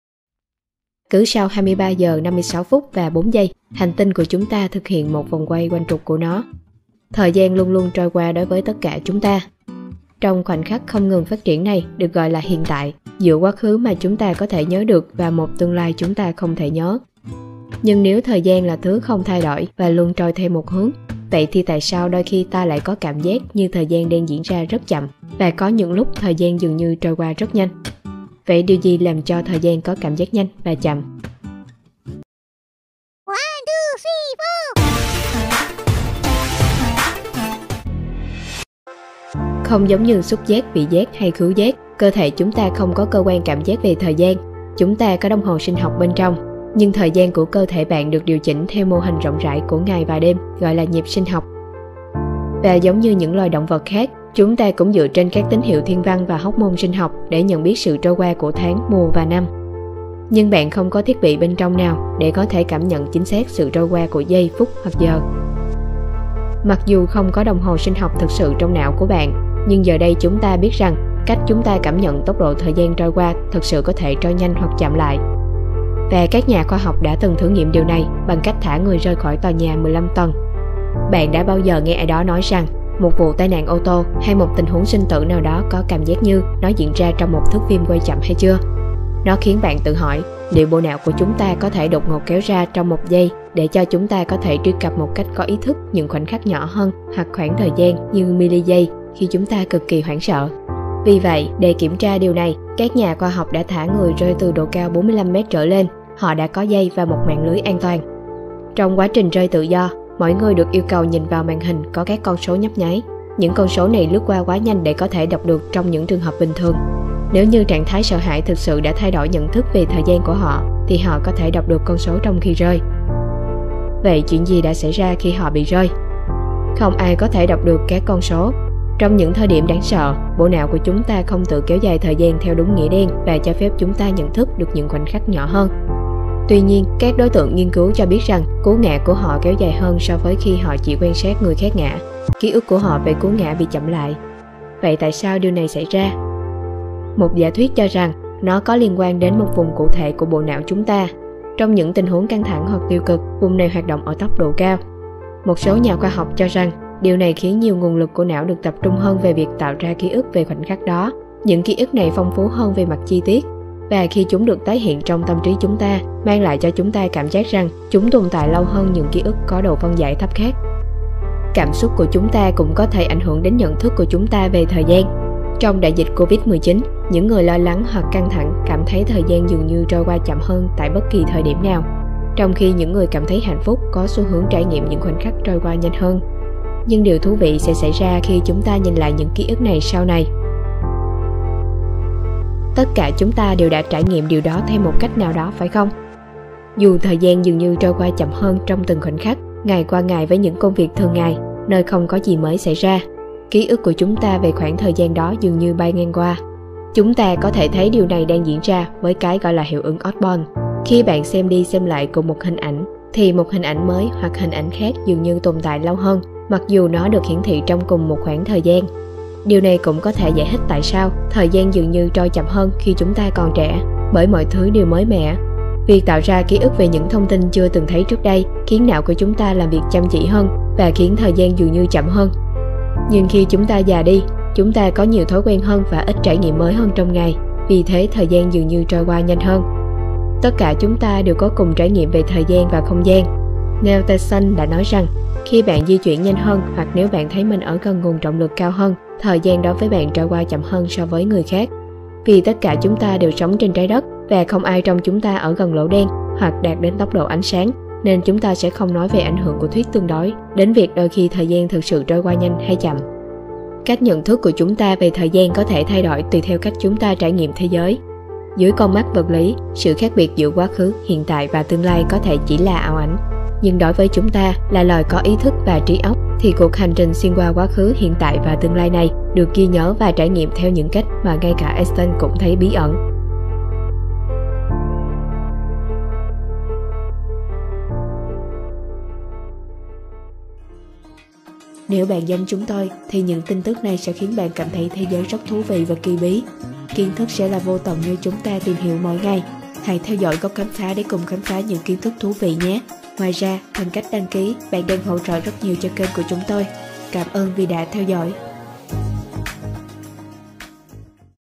Bộ não của bạn khiến thời gian trôi nhanh hay chậm như thế nào???
Cứ sau 23 giờ 56 phút và 4 giây, hành tinh của chúng ta thực hiện một vòng quay quanh trục của nó. Thời gian luôn luôn trôi qua đối với tất cả chúng ta. Trong khoảnh khắc không ngừng phát triển này được gọi là “hiện tại”, giữa quá khứ mà chúng ta có thể nhớ được và một tương lai mà chúng ta không thể nhớ.
Nhưng nếu thời gian là thứ không thay đổi, luôn trôi theo một hướng. Vậy thì tại sao đôi khi ta lại có cảm giác như thời gian đang diễn ra RẤT… CHẬM?! Và có những lúc thời gian dường như trôi qua rất nhanh.
Vậy điều gì làm cho thời gian có cảm giác nhanh và chậm?
Một số video khác về khám phá bạn xem thử:
- Bí ân về "Dark DNA" - phát hiện khiến chúng ta phải xem lại quan điểm về sự tiến hóa